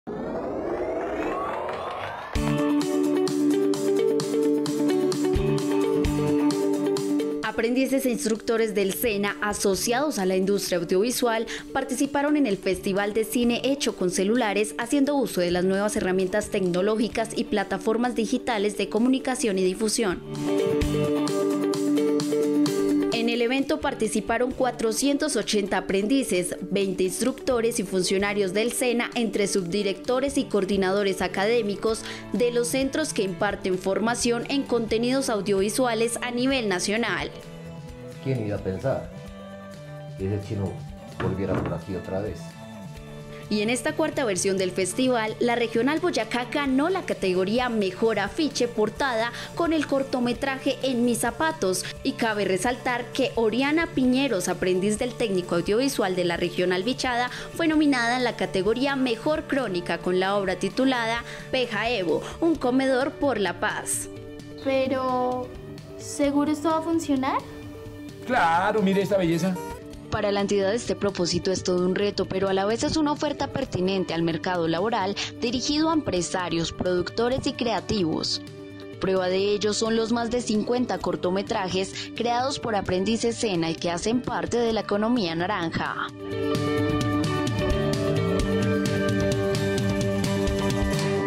SENA TV - Clausura Smartfilms 2018
Nuestra entidad estuvo presente en la premiación de Smartfilms, un festival audiovisual hecho con celulares en donde ademas lograron una nominación y un premio.